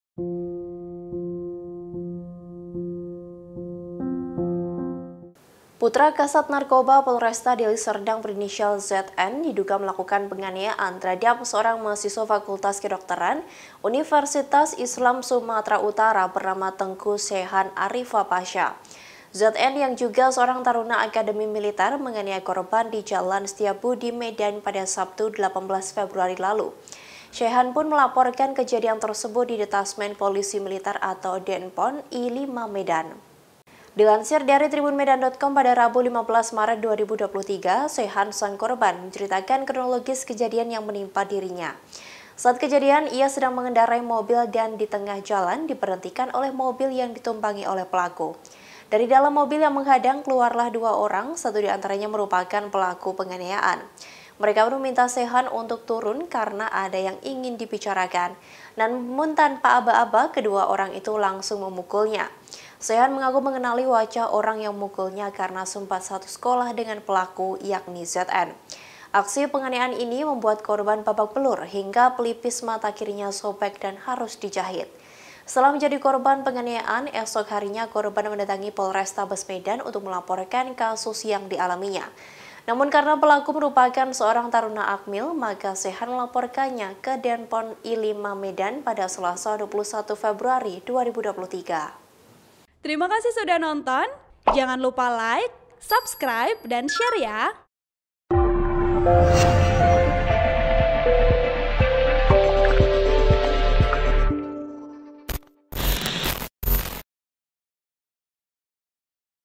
ANAK PEJABAT BERULAH LAGI! Putra Perwira Polisi di Deliserdang Aniaya Mahasiswa hingga Babak Belur
TRIBUN-VIDEO.COM - Putra Kasar Narkoba Polresta Deliserdang berinisial ZN diduga melakukan penganiayaan terhadap seorang mahasiswa Fakultas Kedokteran Universitas Islam Sumatra Utara (UISU), bernama Teuku Shehan Arifa Pasha.

ZN yang juga seorang taruna Akademi Militer (Akmil) menganiaya korban di Jalan Setiabudi, Medan, Sabtu (18/2/2023).

Shehan pun melaporkan kejadian tersebut di Detasemen Polisi Militer (Denpom) I/5 Medan.